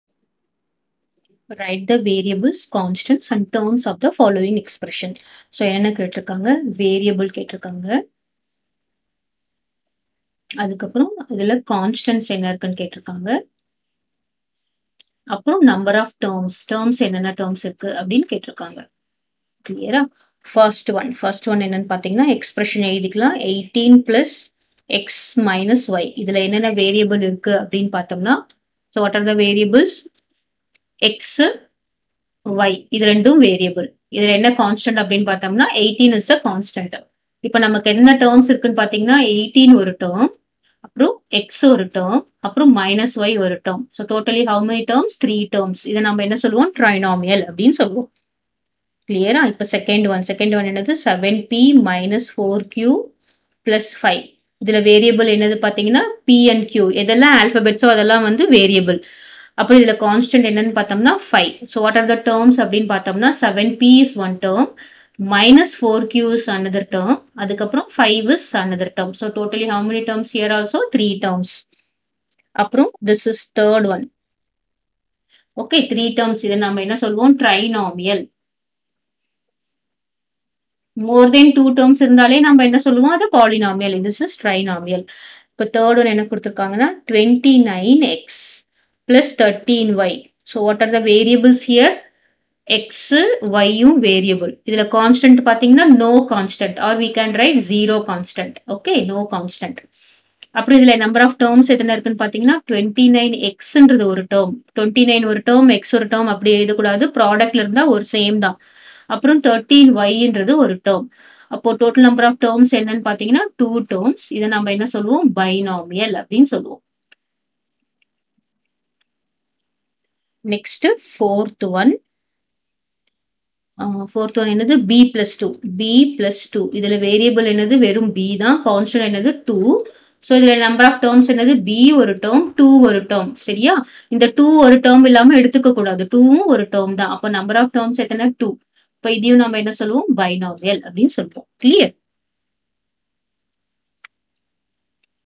Class 7 TN Maths Term I 3.ALGEBRA Exercise 3.1 4. Write the variables, constants and terms of
Term I 3.ALGEBRA
Exercise 3.1
4. Write the variables, constants and terms of the following expressions.
(i)18 + x − y
(ii) 7p − 4q + 5
(iii) 29x + 13y
(iv) b + 2